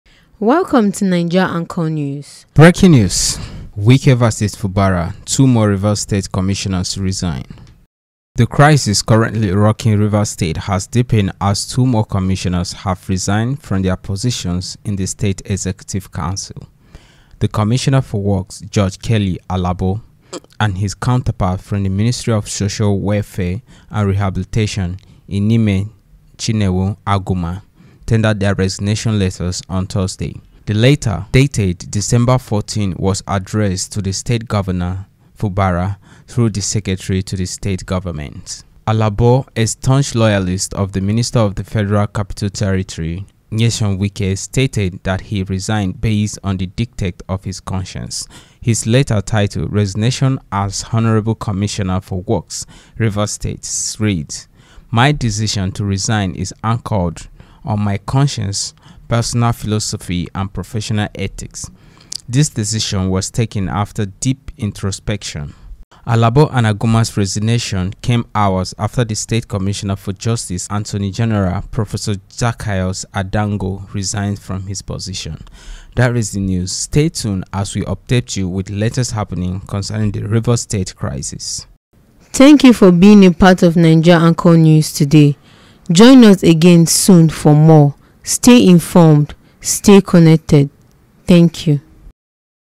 Breaking| Wike Vs Fubara:- 2 Rivers Commissioner Resigns as the saga continues.
BREAKING:  Wike vs Fubara: Two more Rivers Commissioners resign.  The crisis currently rocking Rivers State has deepened as two more commissioners have resigned from their positions in the state executive council.

The Commissioner for Works, George-Kelly Alabo, and his counterpart from the Ministry of Social Welfare and Rehabilitation, Inime Chinwenwo-Aguma, tendered their resignation letters on Thursday.

The letter, dated December 14, was addressed to the state governor, Siminalayi Fubara, through the Secretary to the State Government.  The crisis currently rocking Rivers State has deepened as two more commissioners have resigned from their positions in the state executive council.

The letter, dated December 14, was addressed to the state governor, Siminalayi Fubara, through the Secretary to the State Government.

Alabo, a staunch loyalist of the Minister of the Federal Capital Territory (FCT), Nyesom Wike, stated that he resigned based on the dictates of his conscience.

His letter, titled ‘Resignation as Honourable Commissioner for Works, Rivers State,’ reads: “My decision to resign is anchored on my conscience, personal philosophy, and professional ethics. This decision was taken after deep introspection.”

Alabo and Chinwenwo-Aguma’s resignations came hours after the state Commissioner for Justice and Attorney General, Prof Zacchaeus Adangor, resigned from his position. @NaijaAnchorNews